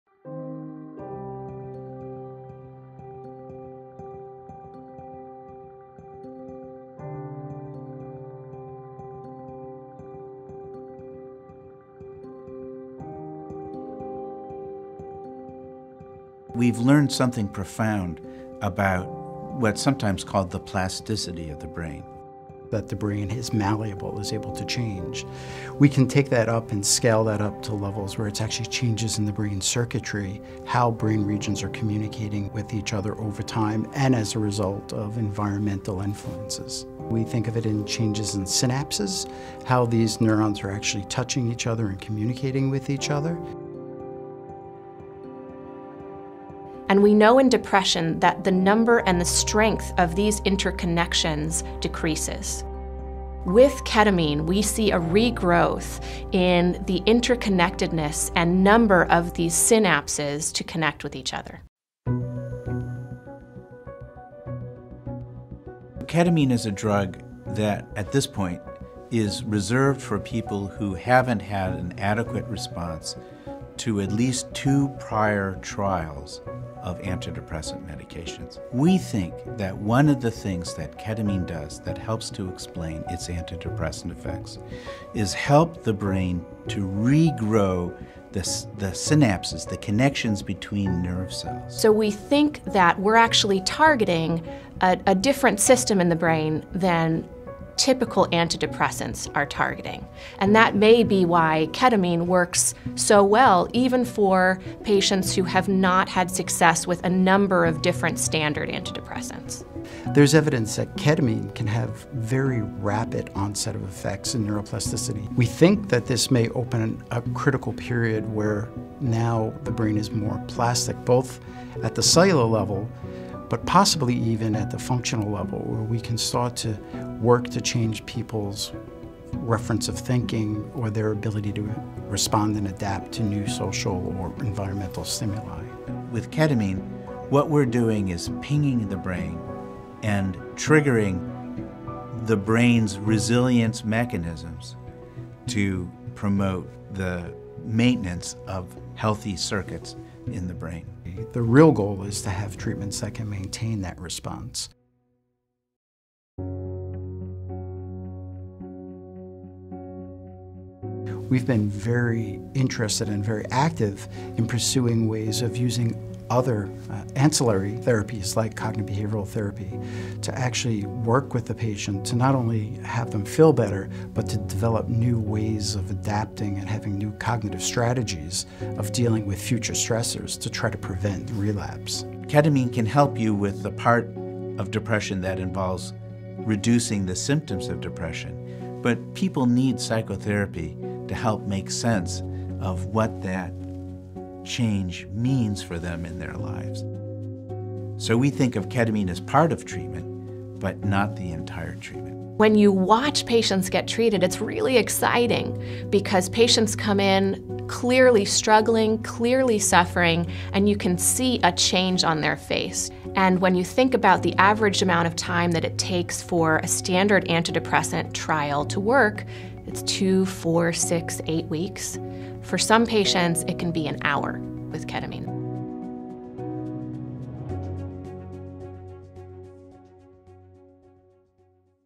Ketamine & Depression: How it Works - Yale Medicine Explains
Depression is one of the most common and most debilitating mental health disorders, affecting some 17 million adults in the US. It also continues to be a misunderstood, often hard-to-treat illness. Researchers have worked for decades to better understand the neurobiology underpinning depression. For patients with severe, treatment-resistant depression, spending months or even years searching for good treatments can be totally disabling. The prevailing hypothesis for years was that depression was regulated by the neurotransmitter’s serotonin and norepinephrine. Eventually, data began to suggest that maybe something much larger and more global was involved in the brain to account for depression, which led researchers to begin working with glutamate and GABA, the most abundant neurotransmitters in the brain. These chemicals are involved in neuroplasticity – the brain’s ability to adapt to change and protect itself against stressful events. Neuroplasticity is a physical thing, too: it manifests itself “in terms of synapses, how these neurons are actually touching each other and communicating with each other,” explains Gerard Sanacora, PhD, MD, Director of the Yale Depression Research Program. “And we know that in depression, the number and strength of these interconnections decreases,” says Rachel Katz, MD, a professor of Clinical Psychiatry at Yale.  Ketamine – originally developed and still used as an anesthetic – works on those two neurotransmitters and was discovered to have rapid antidepressant effects. Some experience an improvement in symptoms in 24 hours or less. “We think that one of the things that Ketamine does, that helps to explain its antidepressant effects, is help the brain to regrow the synapses, the connections between nerve cells,” says John Krystal, MD, Chair of the Department of Psychiatry at Yale. Because Ketamine seems to work on a totally different system in the brain than typical antidepressants, “that may be why Ketamine works so well, even for patients who have not had success with a number of standard antidepressants,” says Dr. Katz. Ketamine as a treatment for depression gained FDA approval last year with the release of Spravato (esketamine), and for now, it remains a treatment reserved for patients who haven’t found success with at least two other antidepressant treatments. Dr. Sanacora explains that other treatments, including cognitive behavioral therapy, are important to use in conjunction with ketamine, “to develop new ways of adapting and having new cognitive strategies of dealing with future stressors to prevent relapse.” New therapies like ketamine are changing how depression is understood, and may pave the way for future research that helps us understand the brain in all of its complexity.